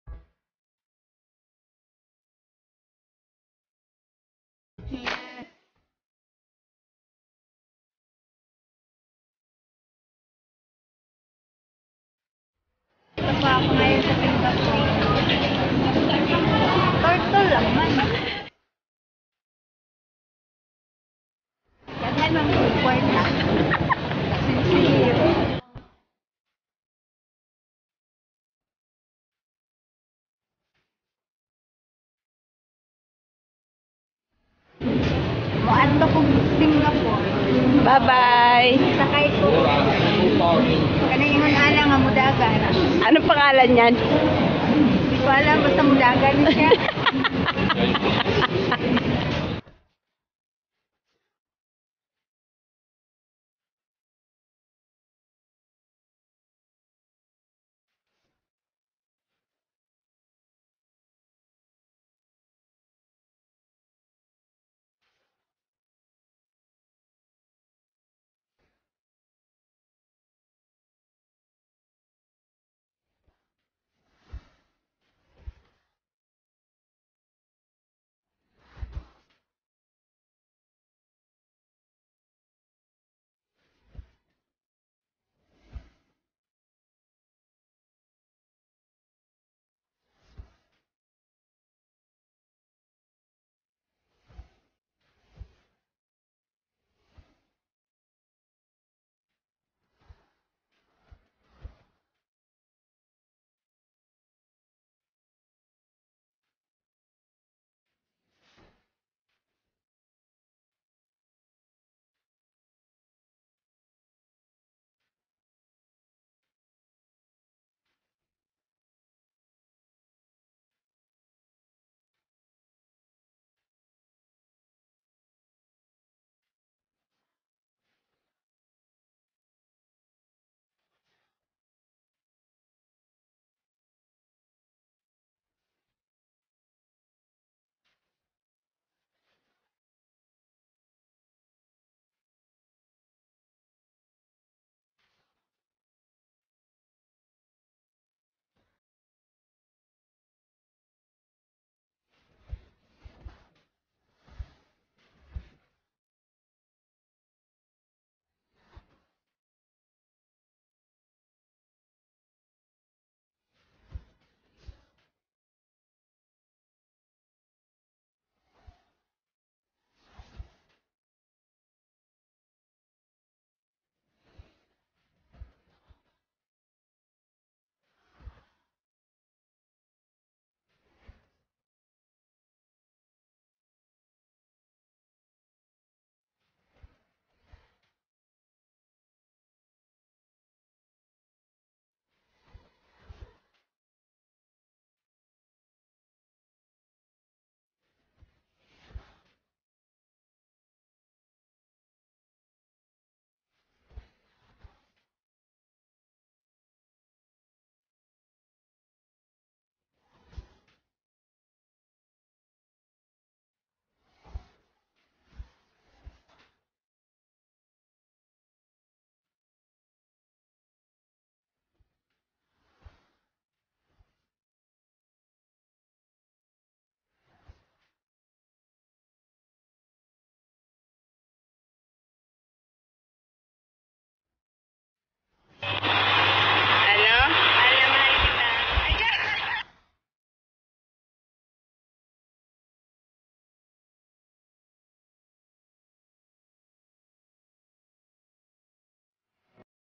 Happy Birthday Love 😻 (Oct26.2019)
Birthday party of my kuting
Simple celebration
More birthdays to come beibie